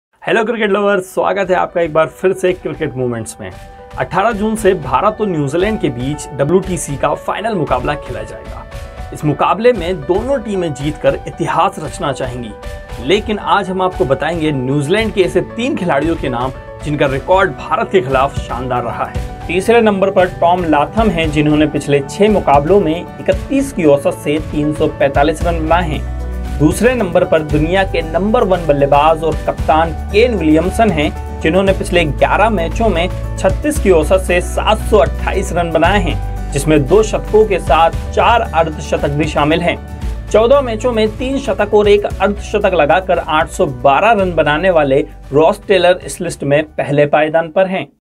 WTC FINAL , INDvsNZ: Most Successful New Zealand Batsman against India in Test
In this video we talk about 3 most successful New Zealand batsman against India in Test.
1. Kane Williamson
2. Ross Taylor
3. Tom Latham